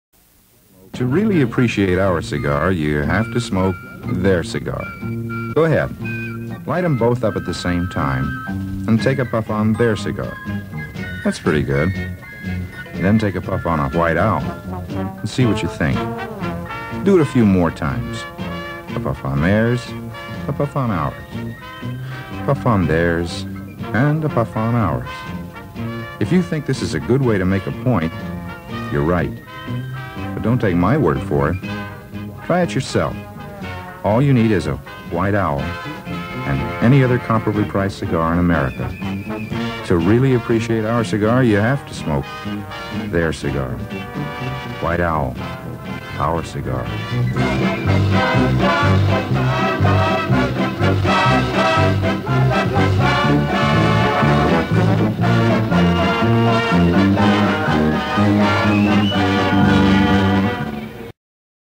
White Owl Cigars
Remember the White Owl commercial " To really Appreciate their Cigar you've got to smoke Our Cigar"?